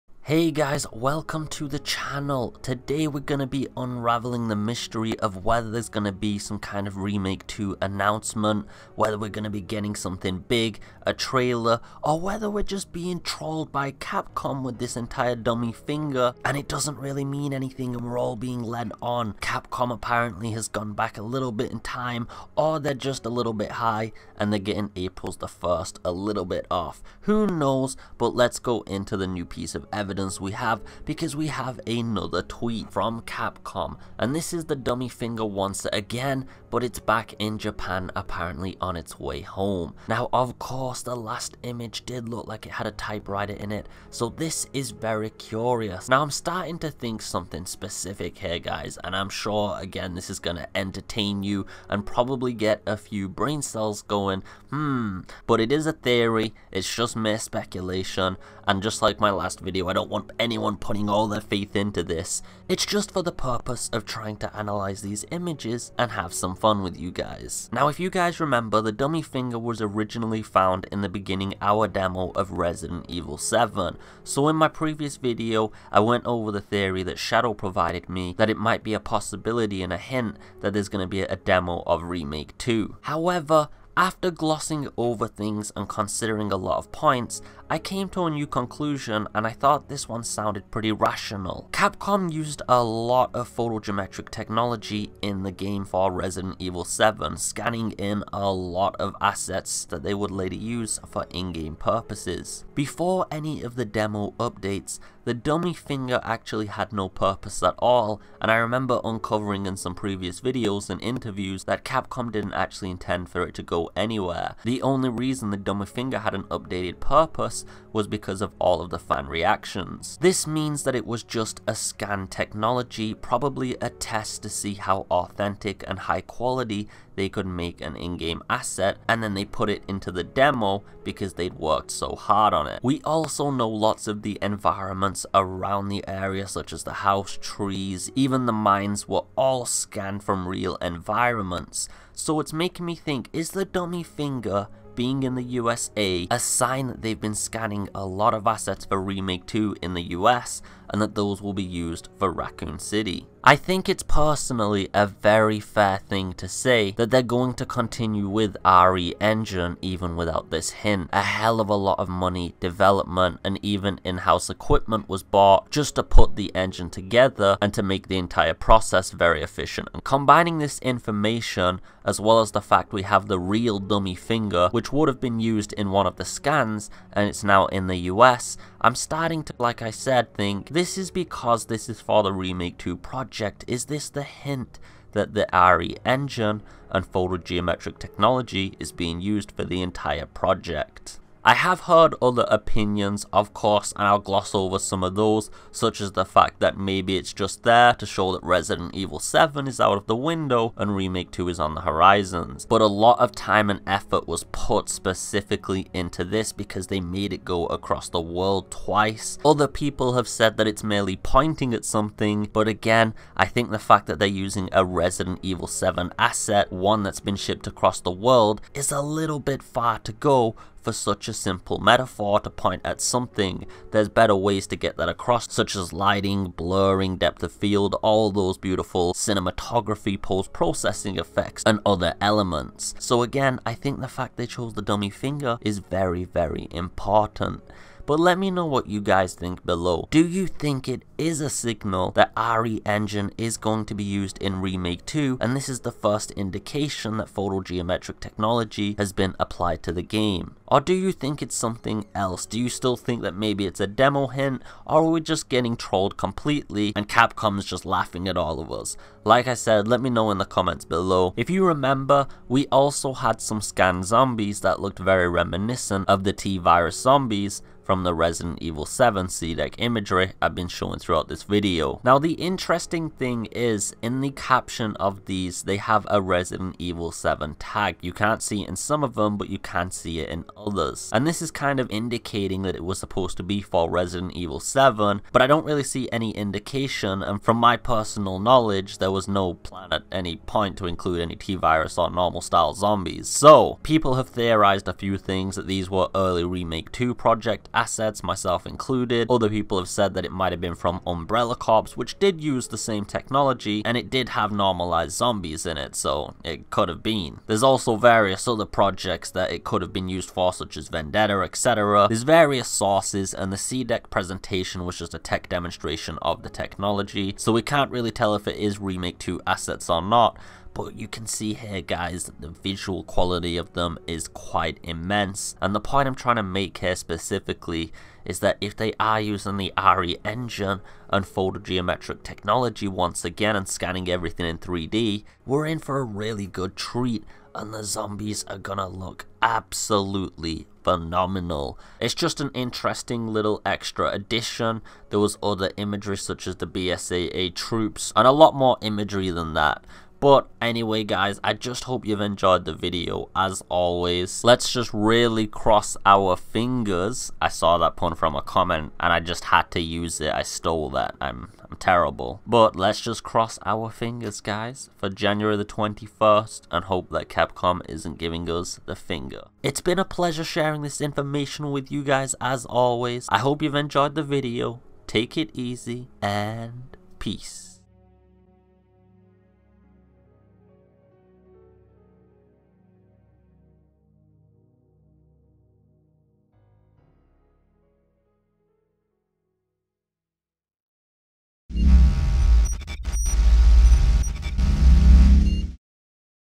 Resident Evil 2 Remake Secret Twitter Puzzle, WHAT IS IT? ANOTHER PIECE.
We get another Tweet from the Team over at Capcom, wonder what the hell they are up to, clearly something about Resident Evil 2 Remake